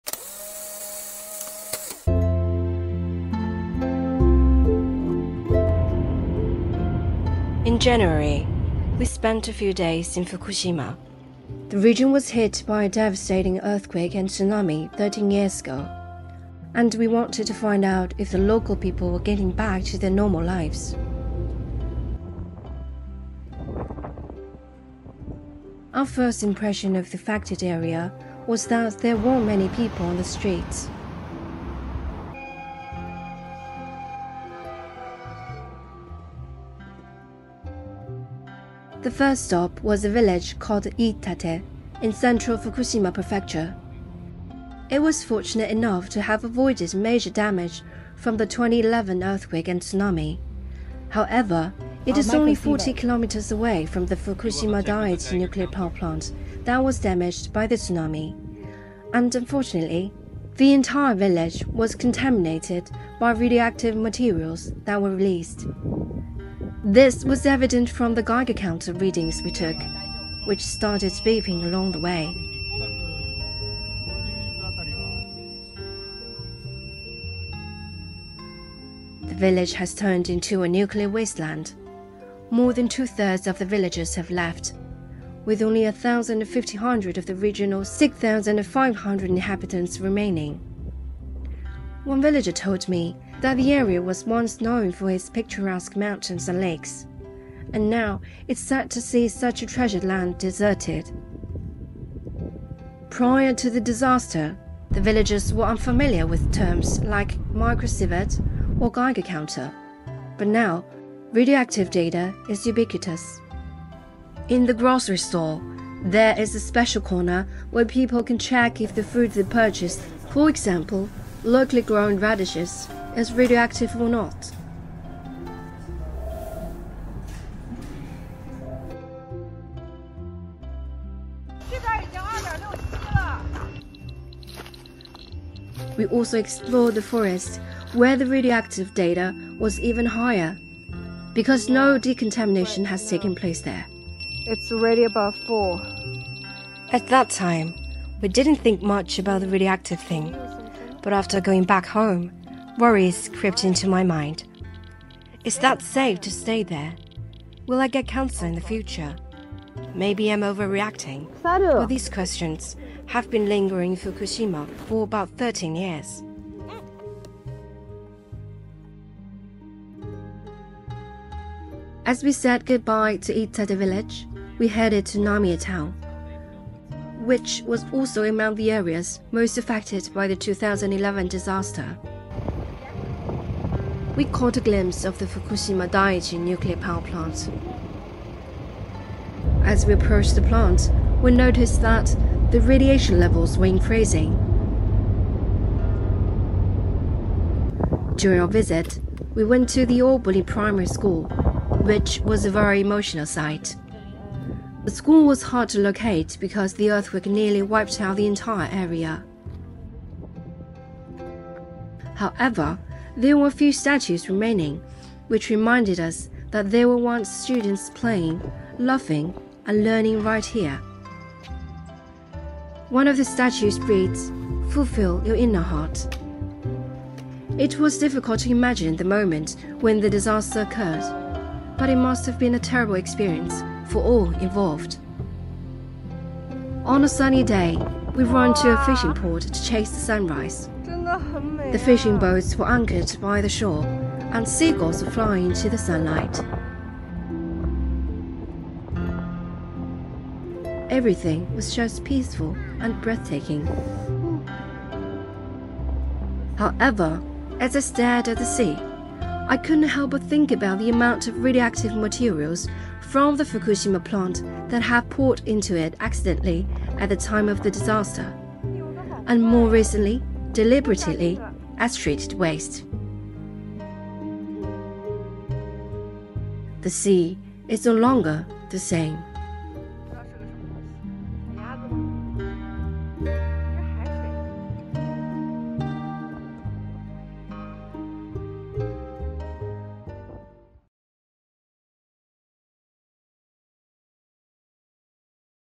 Vlog | Diario sobre Fukushima: aún a la sombra del desastre
En enero pasamos unos días en Fukushima. La región sufrió un devastador terremoto y tsunami hace 13 años, y queríamos saber si la población local estaba volviendo a su vida normal.
Mientras contemplaba el mar, no pude evitar pensar en la cantidad de materiales radiactivos de la central de Fukushima que se han vertido en él accidentalmente en el momento de la catástrofe, y más recientemente, deliberadamente, como residuos contaminados. El mar ya no es el mismo.